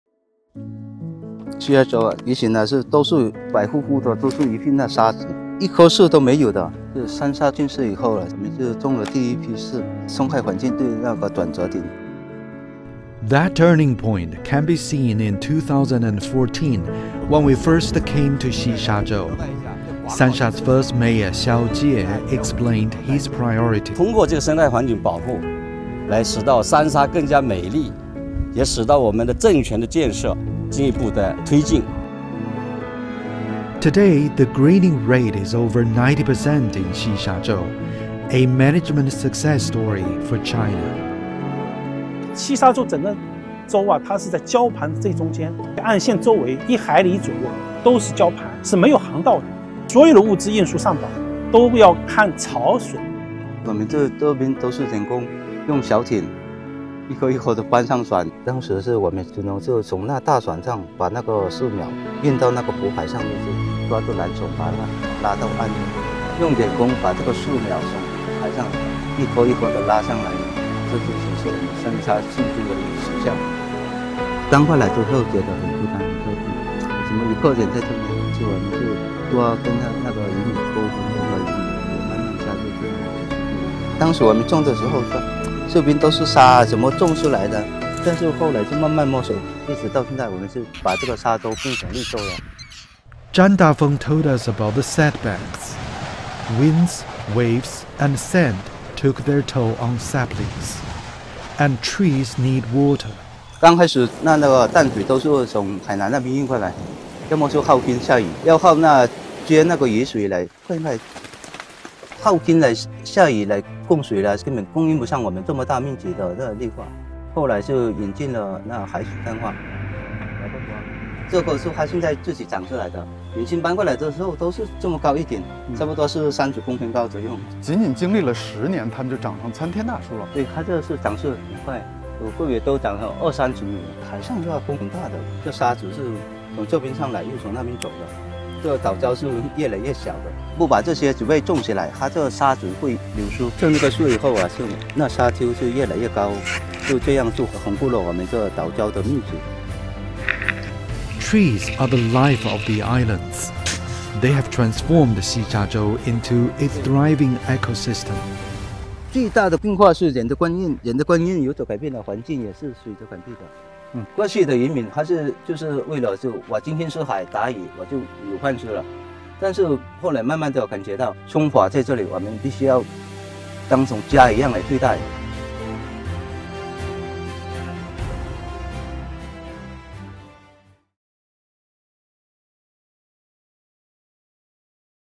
Reverdeciendo el azul | Xishazhou enverdece
En el corazón del mar Meridional de China se encuentra Xishazhou. Este rincón fronterizo estéril ahora tiene un índice de ecologización de más de 90 %, lo que supone una historia de éxito de la gestión ecológica en China.